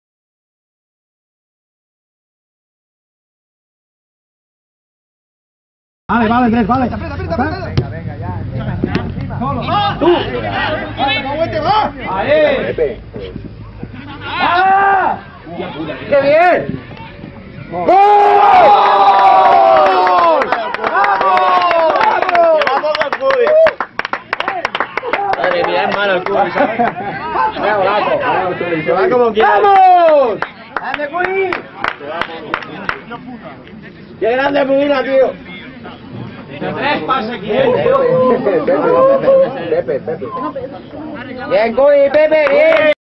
J15 - 1-1 Copoví.mp4
1er gol de l'Òdena durant el partit del Campionat de lliga de futbol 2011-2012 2ª Catalana corresponent a l'enfrontament La Munia-Òdena - 8/01/2012 - Jornada 15